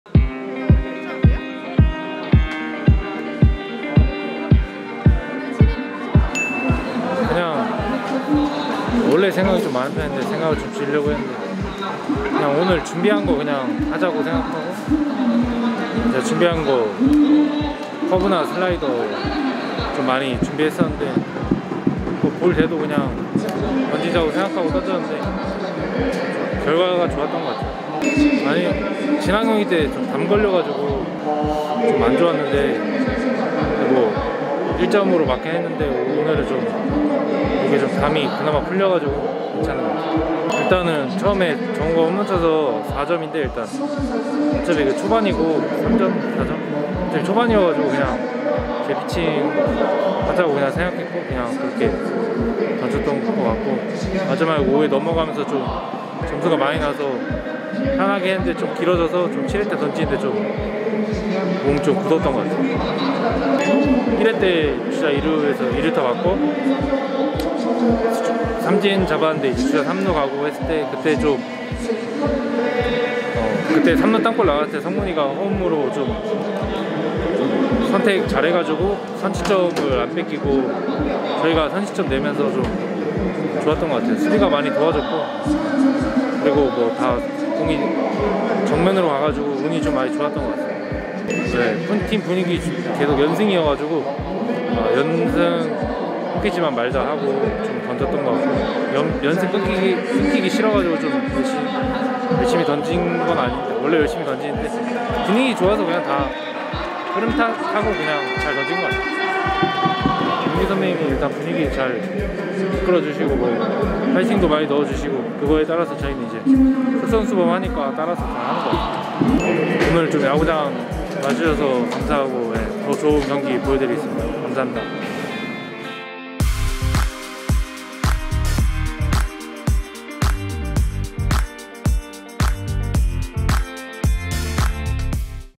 최원태 7이닝 3피안타 5탈삼진 무사사구 무실점 완벽투 그대의_브라운아이즈에_치얼스🥂 | Heroes in Heroes (4.12)
원래도 열심히 던지지만 오늘 더 열심히 던진 최고급커피😘
그대의 브라운아이즈에 치얼스🥂